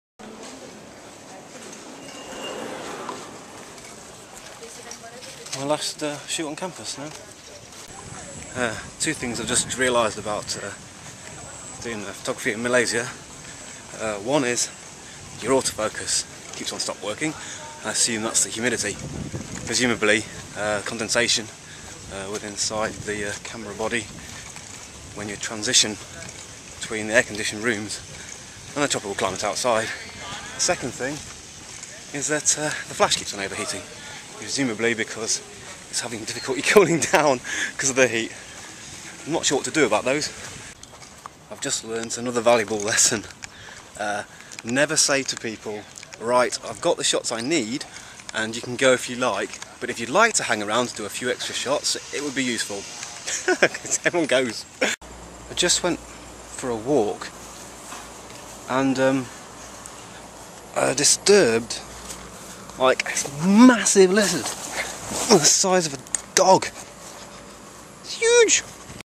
Malaysia photography trip vlog day 7
Day 7 with Malaysia photography trip vlog. Problems with photography in Malaysia. Last days shoot on campus.